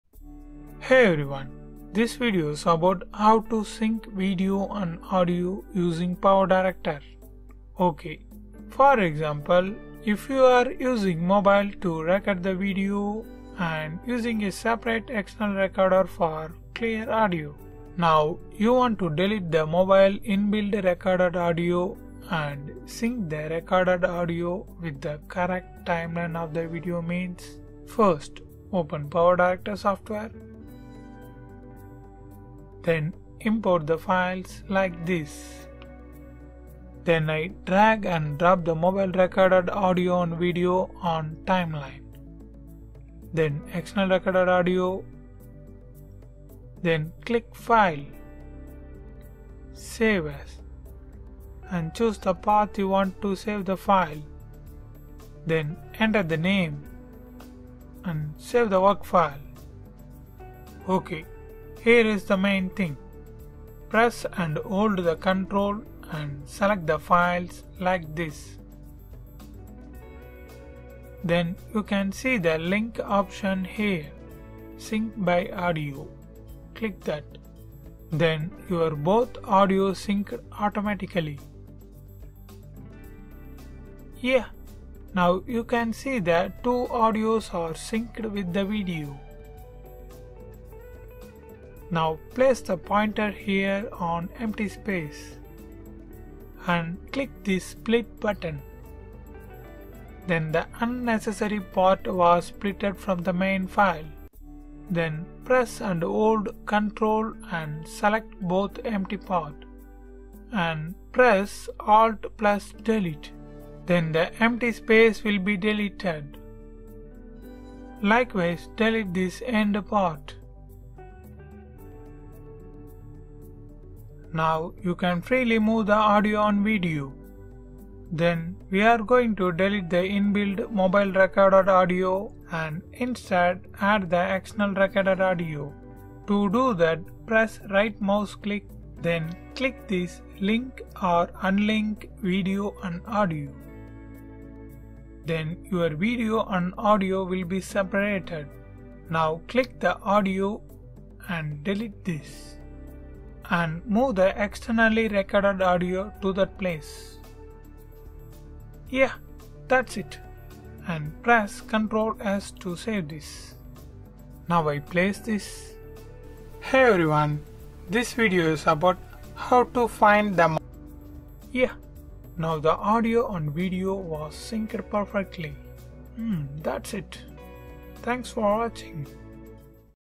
How to Sink Audio and Video on Power Director at One Click? | Step by Step
How to Sink Audio and Video on Power Director at One Click? In this video i will showed about how to sink any audio and video using power director software by just single click. Now any one can easily sink the different photages audio and video using this simple trick.